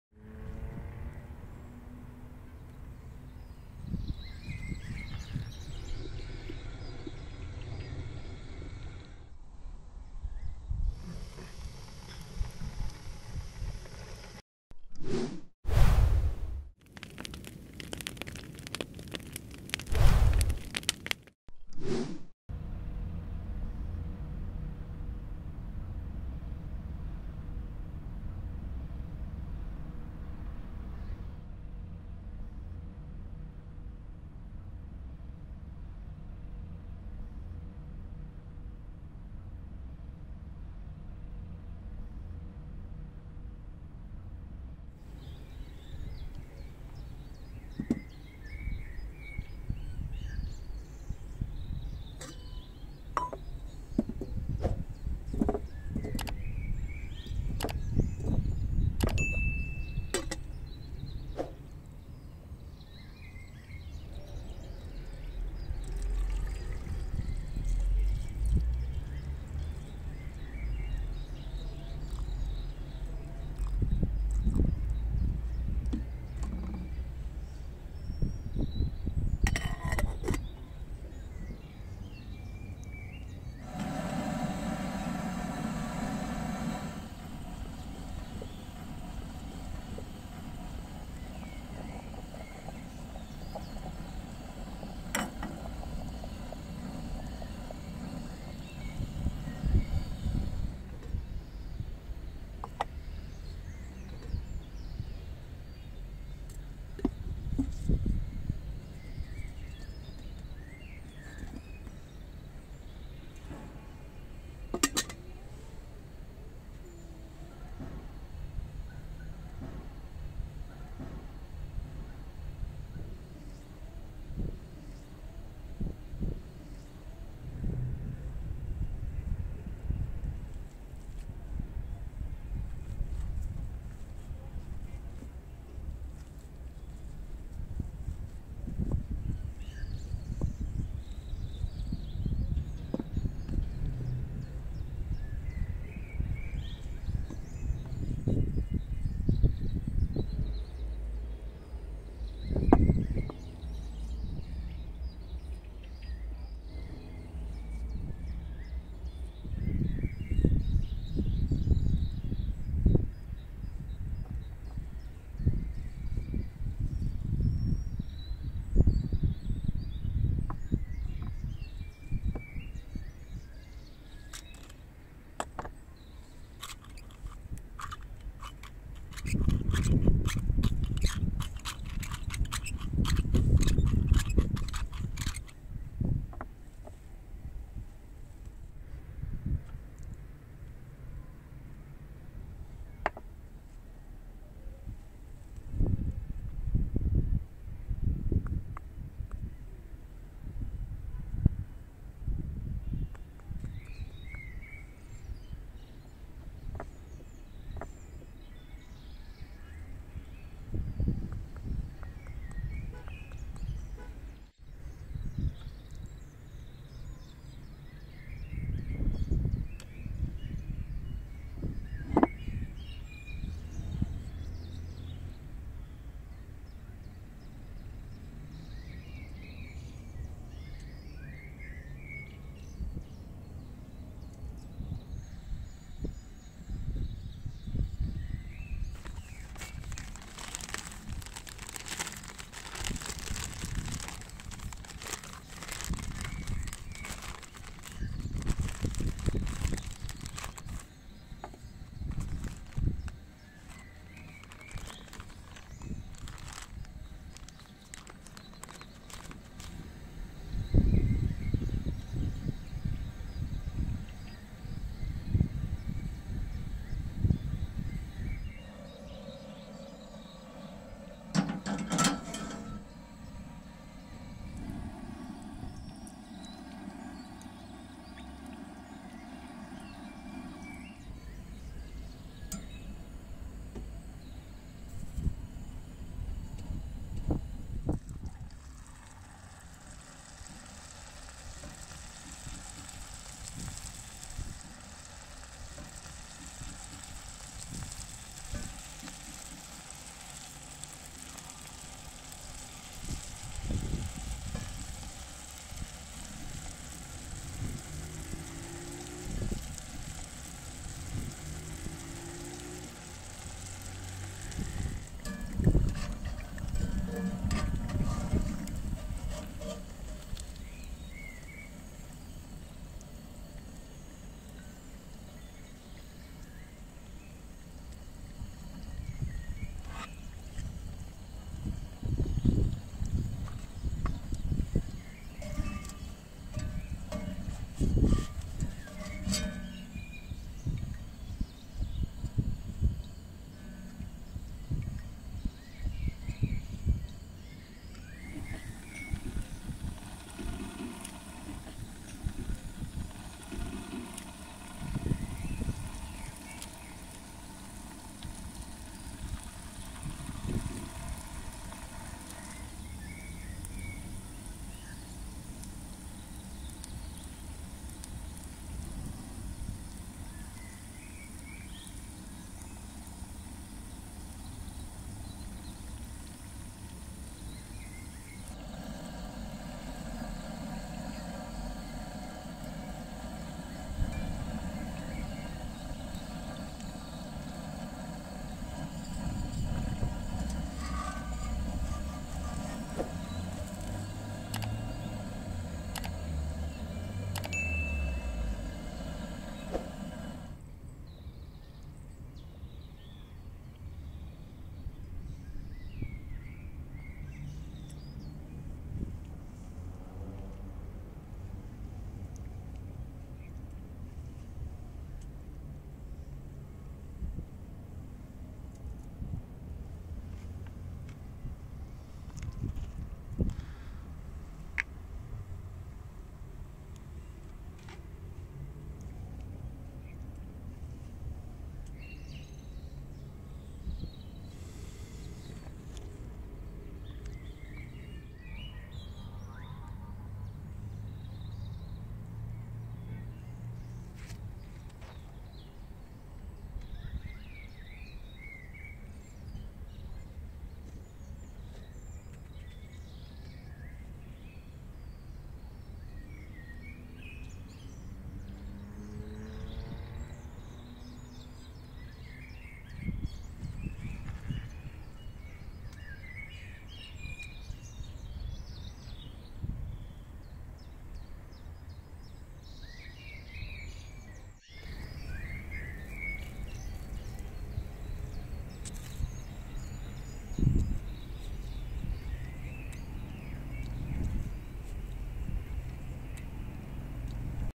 (22) طريقة عمل بطاطس مقلية سناكس بطريقة ع_الشواية - how to make fries for snacks
النهاردة البطاطس المحمرة ليها طريقة جديدة .. سناكس على السريع ... ليها قرمشة من بره ودايبة من جوه